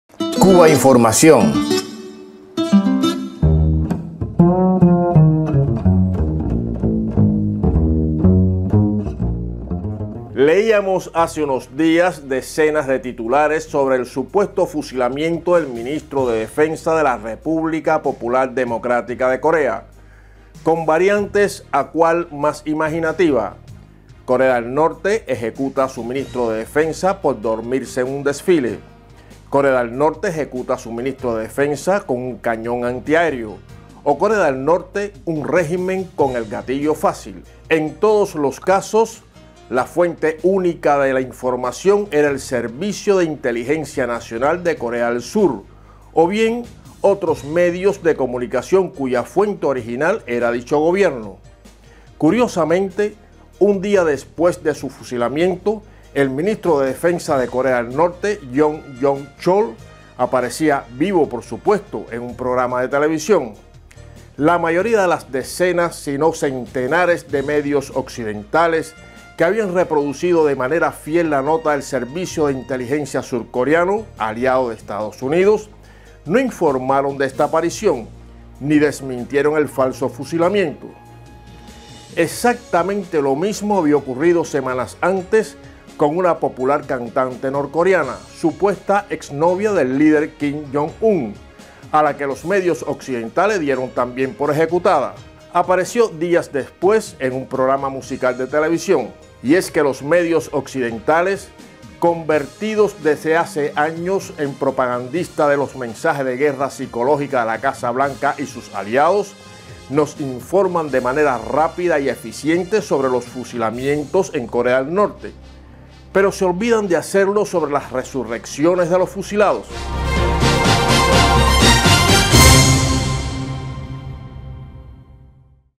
Corea del Norte: medios occidentales informan de fusilamientos, pero no de las resurrecciones
Cubainformación TV – Basado en un texto publicado en Tercera Información.- Leíamos hace unos días decenas de titulares sobre el supuesto fusilamiento del ministro de Defensa de la República Popular Democrática de Corea, con variantes a cual más imaginativa: “Corea del Norte ejecuta a su ministro de Defensa por dormirse en un desfile”, “Corea del Norte ejecuta a su ministro de Defensa con un cañón antiaéreo”, o “Corea del Norte, un régimen con el gatillo fácil”. Edición: Esti Bonito. Fuente